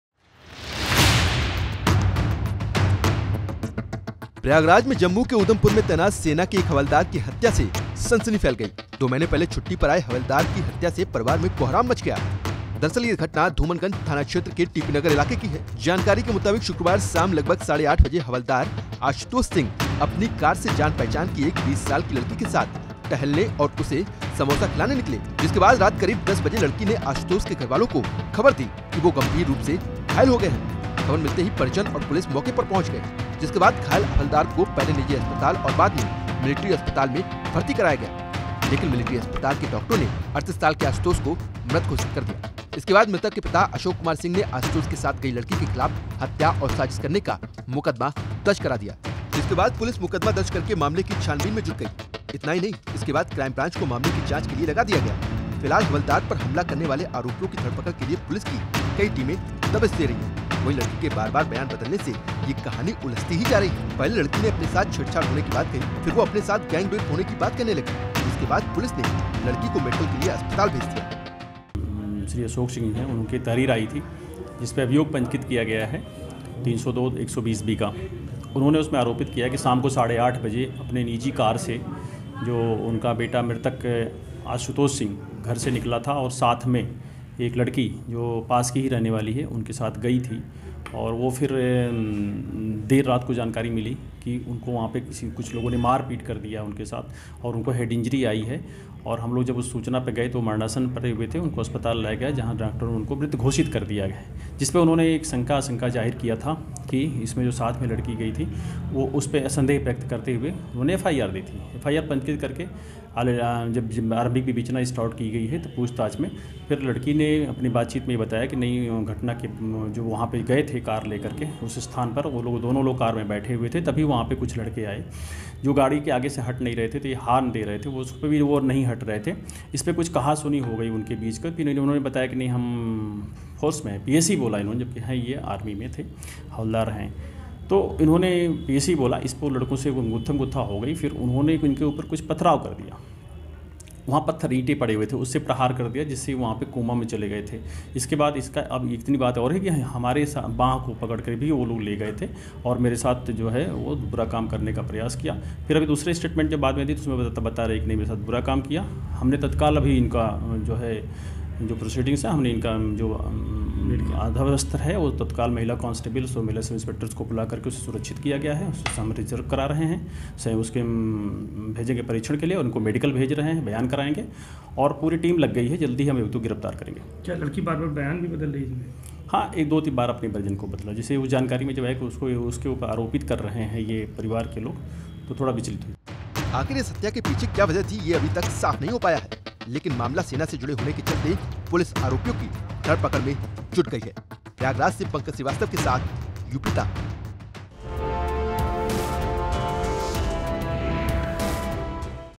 Prayagraj में सेना के जवान की हत्या से पुलिस प्रशासन में मचा हड़कंप!
संगम नगरी प्रयागराज में जम्मू के उधमपुर में तैनात सेना के एक हवलदार की हत्या से सनसनी फैल गई है. सेना के हवलदार की हत्या से परिवार में कोहराम मचा हुआ है.